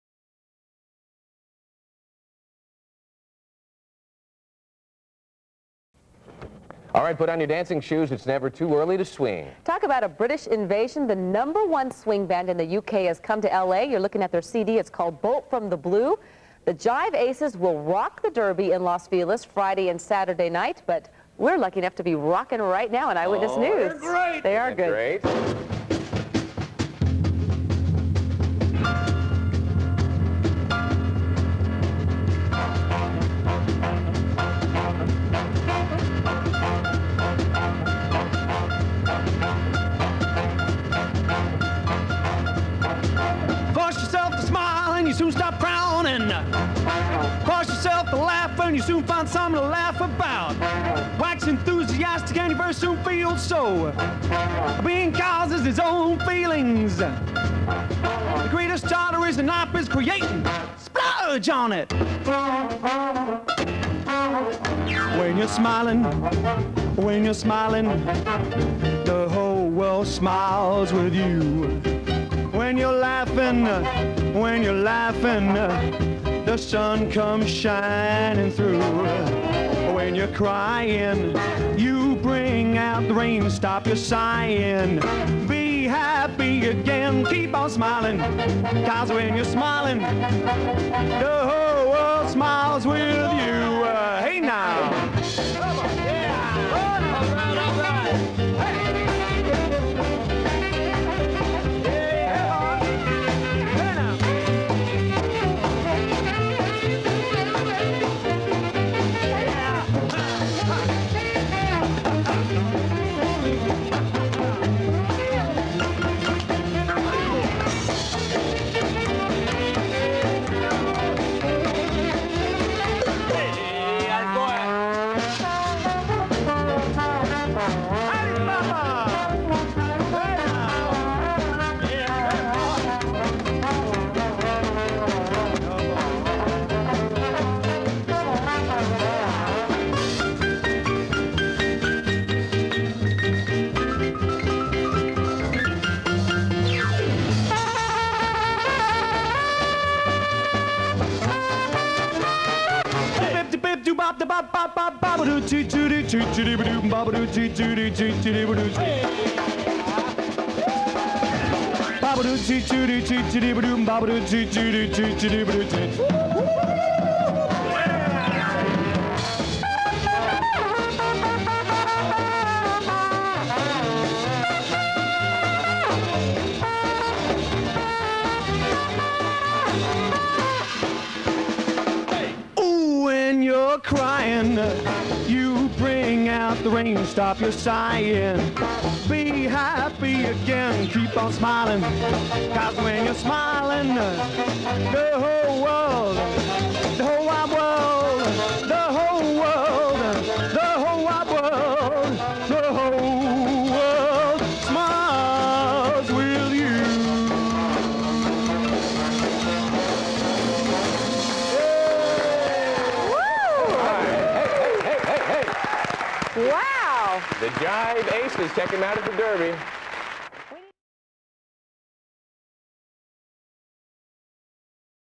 The Aces Vintage Vault - ABC TV Los Angeles (1998)
Please take our short live music survey, we'd love to get your opinion! Promoting one of our first ever performances at The Derby in L.A. on ABC 7 TV in Los Angeles. Just look at how early we had to get up to do this show!